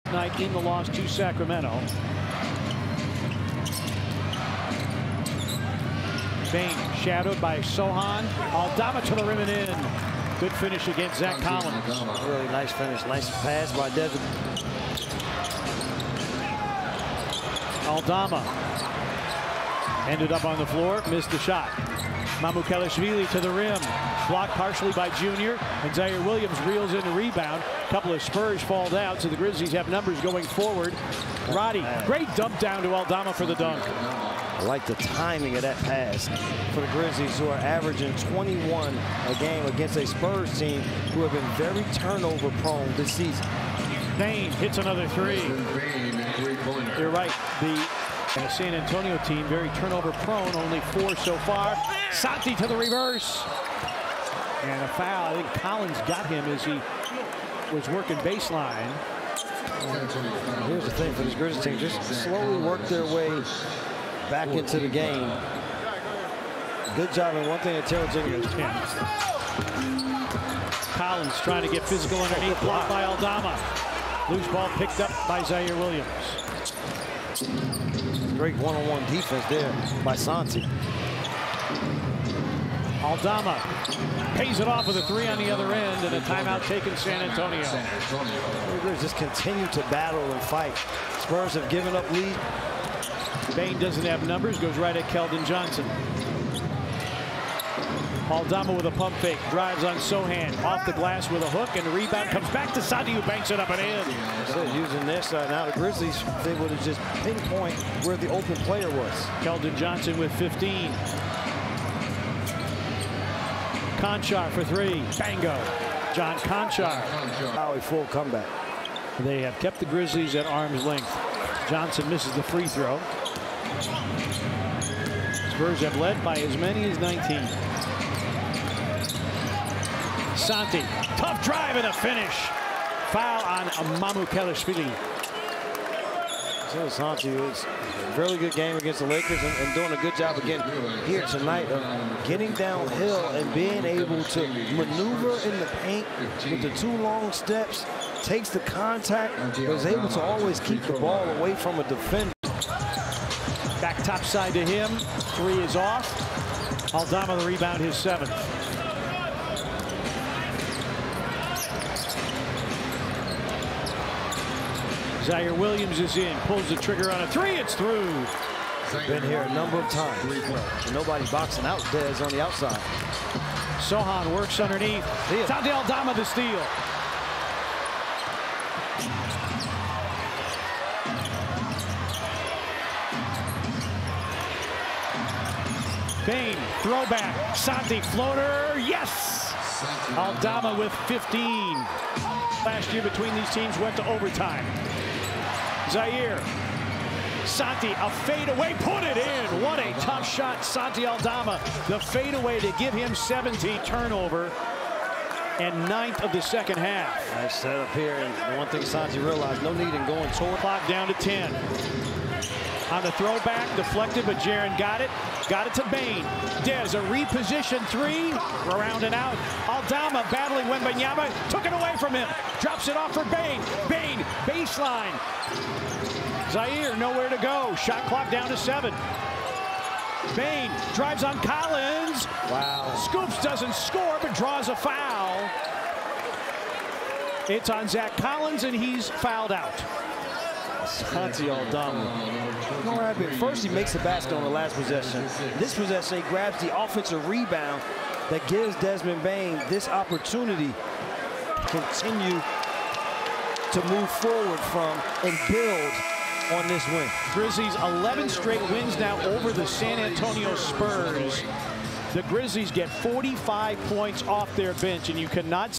Santi Aldama vs Spurs (18 - 11 - 2023)
San Antonio Spurs vs Memphis Grizzlies (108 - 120)
Santi Aldama: 36 min, 17 pts, 10 reb, 3 ast, 1 blk, 7/15 FG, 1/5 3PT, 2/3 FT